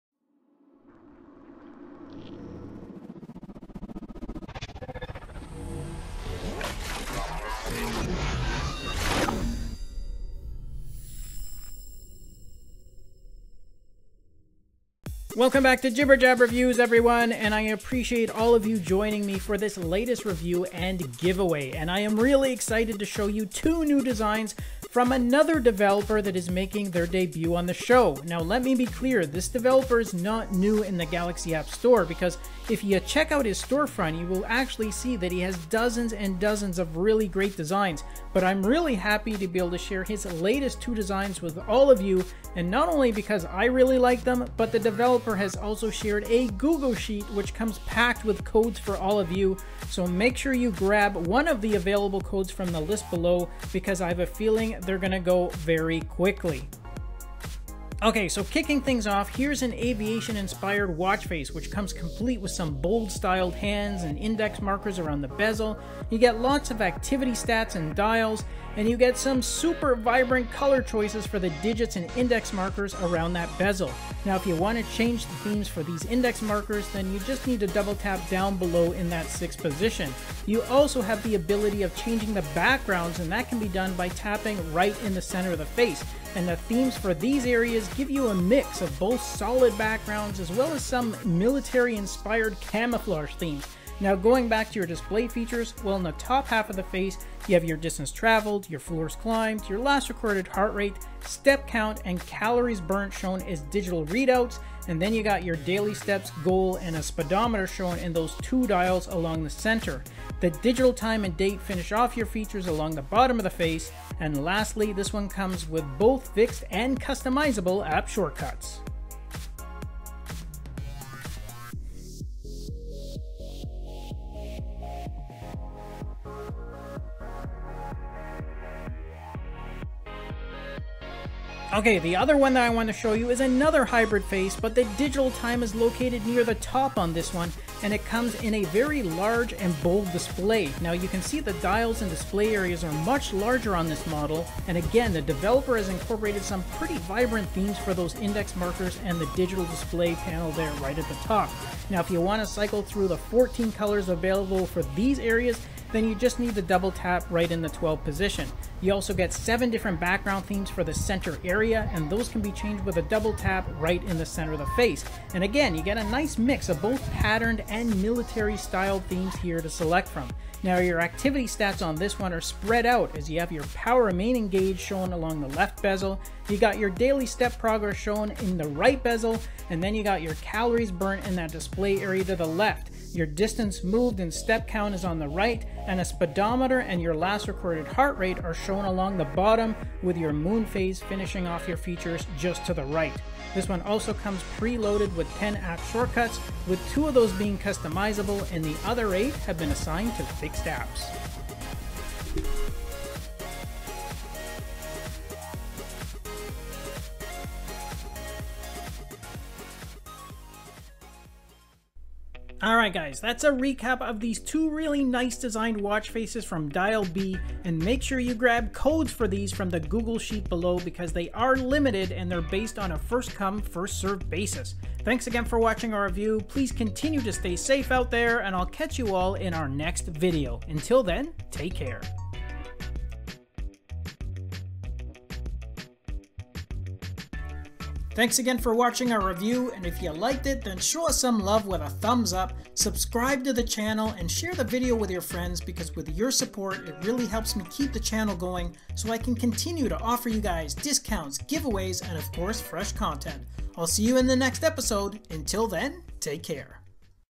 Limited Codes to Giveaway! Samsung Galaxy Watch Active 2/Galaxy Watch Faces by Dialbee
Here are two very fresh and modern designs from this developer that you can add to your collections for free by picking up an available code from the Google Sheets below.  Note: codes are on a first come, first use basis!

Samsung Watchfaces
Smartwatch